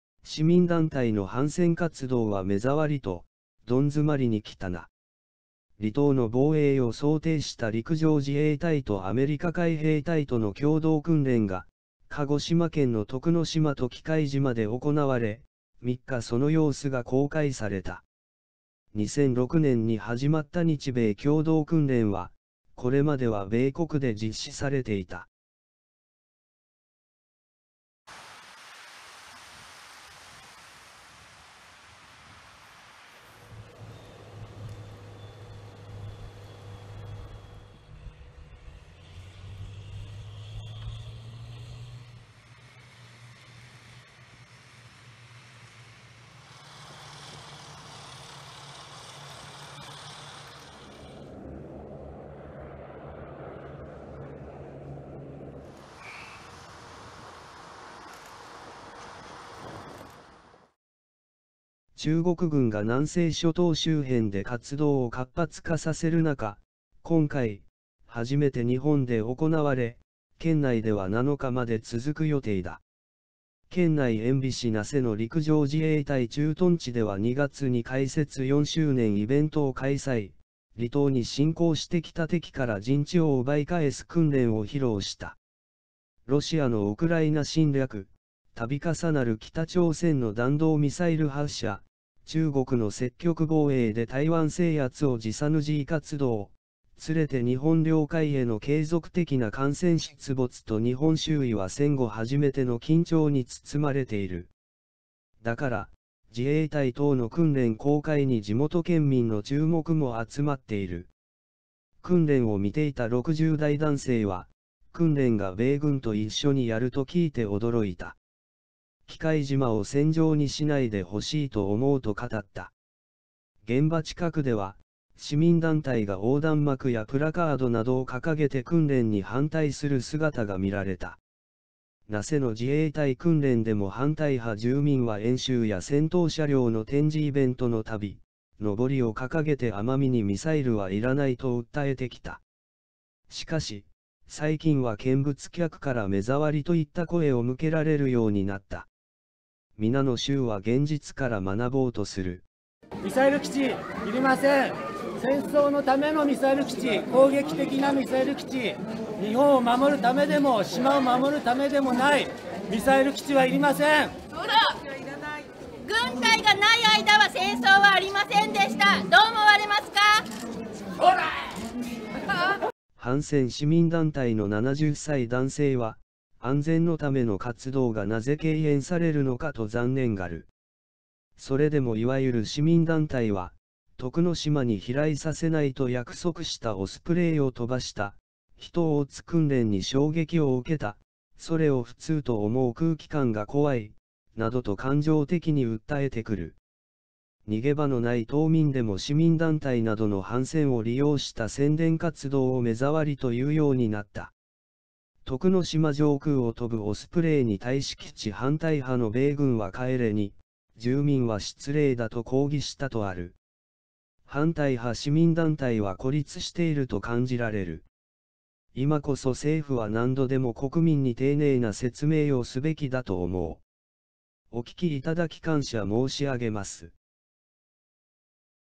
市民団体の反戦活動は「目障り」と…どん詰まりに来たな
離島の防衛を想定した陸上自衛隊とアメリカ海兵隊との共同訓練が、鹿児島県の徳之島と喜界島で行われ、３日その様子が公開された。2006年に始まった日米共同訓練は、これまでは米国で実施されていた。